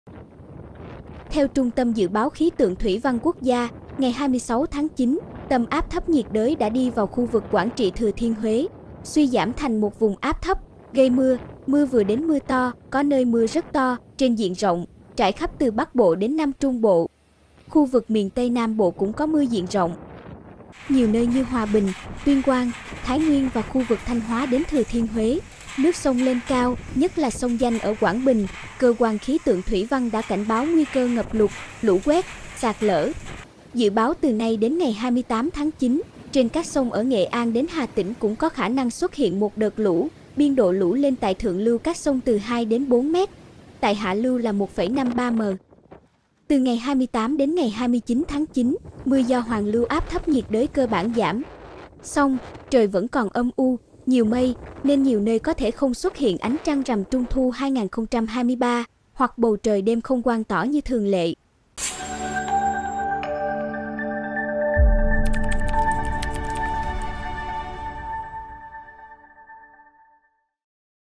Thời tiết xấu, nhiều nơi trời âm u, không thấy trăng rằm
Theo Trung tâm Dự báo khí tượng - thủy văn quốc gia, ngày 26-9, tâm áp thấp nhiệt đới đã đi vào khu vực Quảng Trị - Thừa Thiên Huế, suy giảm thành một vùng áp thấp, gây mưa, mưa vừa đến mưa to (có nơi mưa rất to) trên diện rộng, trải khắp từ Bắc bộ đến Nam Trung bộ. Khu vực miền Tây Nam bộ cũng có mưa diện rộng.